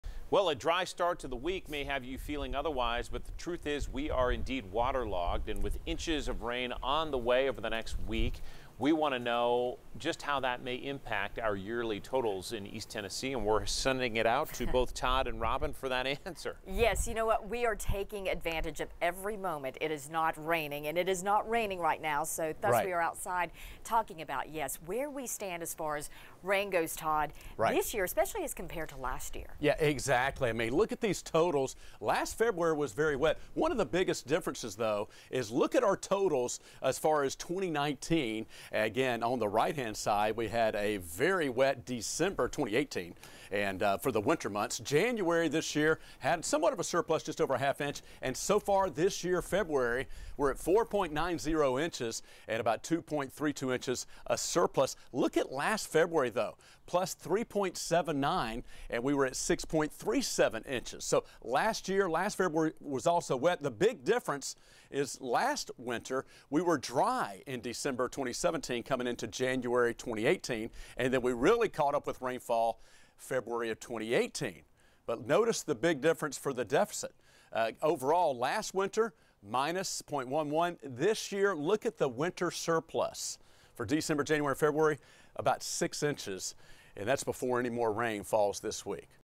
Ask Todd: We're waterlogged
A dry start to the week may have you feeling otherwise, but the truth is, we're waterlogged.
And with inches of rain on the way over the next week... we want to look at how that could impact yearly totals in East Tennessee.